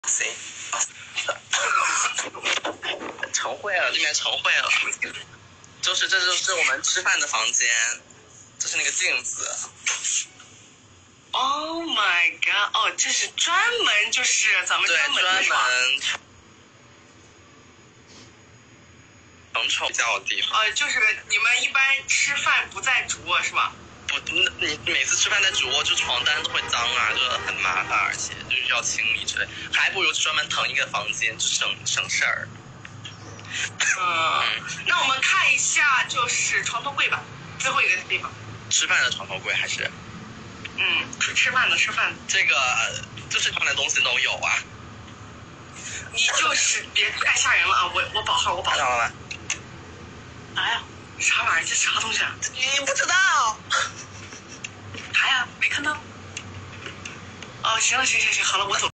[Eng] Eating🔥🔞 front of mirror | gay couple Carlo and XiaoYang
I don't know why I can't  reply your comments can anyone help me

if anyone knows how can comment please

subtitles by carloforever